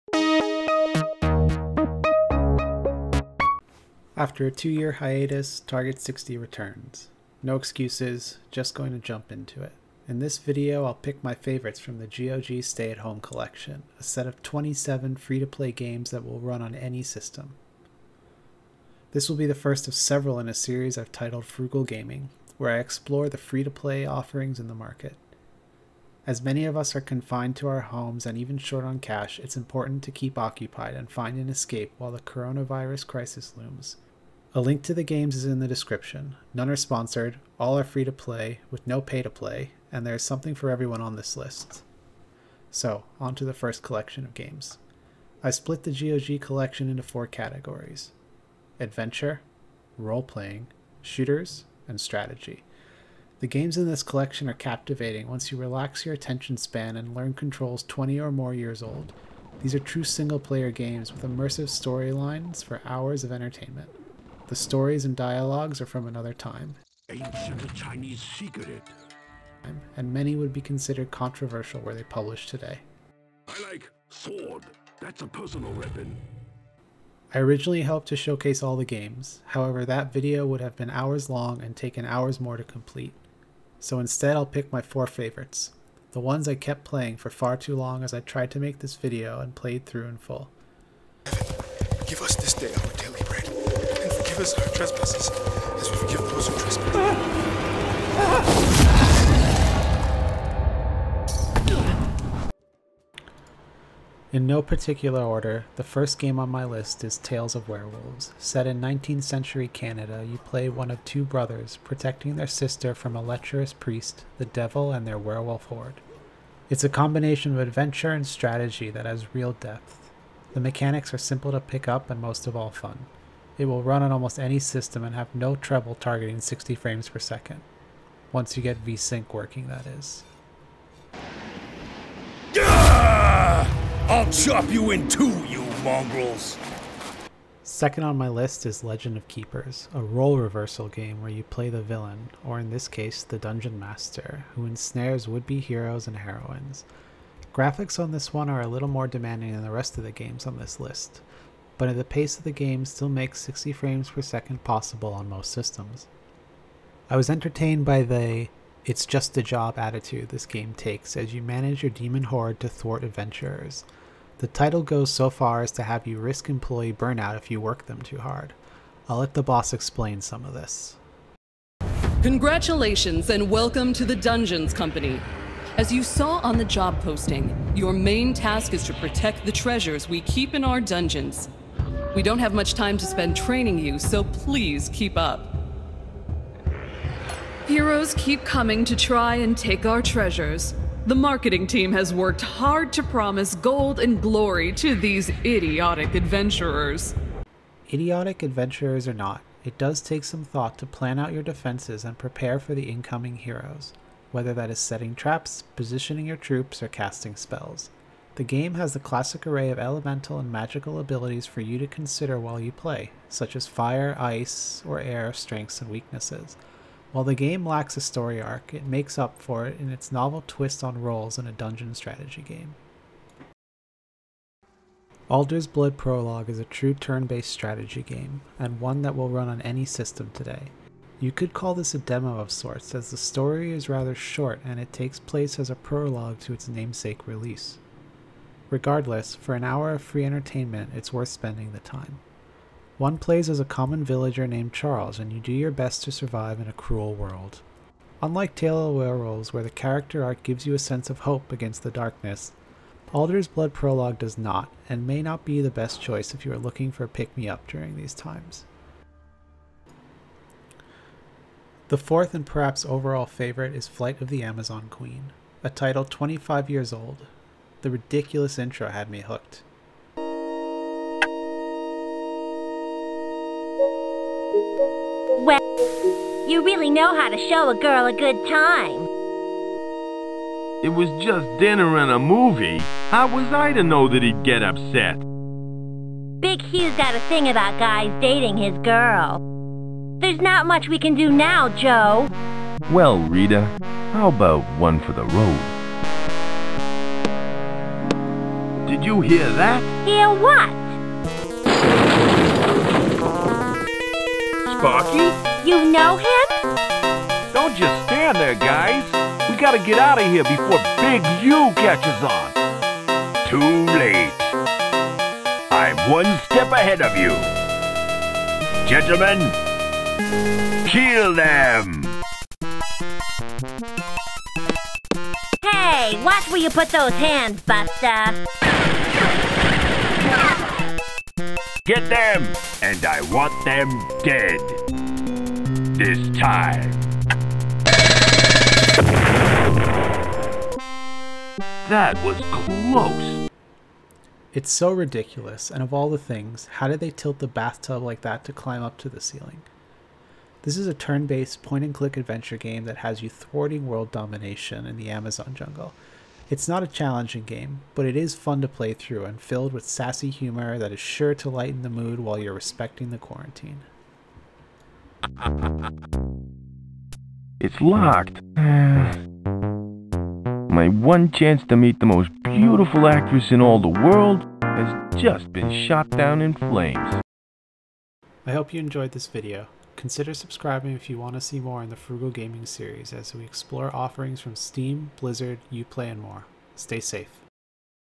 Frugal Gaming - GOG's Free 'Stay @ Home Collection'
Today I take a look at the GOG Stay at Home Collection and pick my favorites from 28 free to play titles that can help pass the time while we're observing the coronavirus quarantine.

 - Early access and exclusive content for patrons

- Sneak peaks and community discussions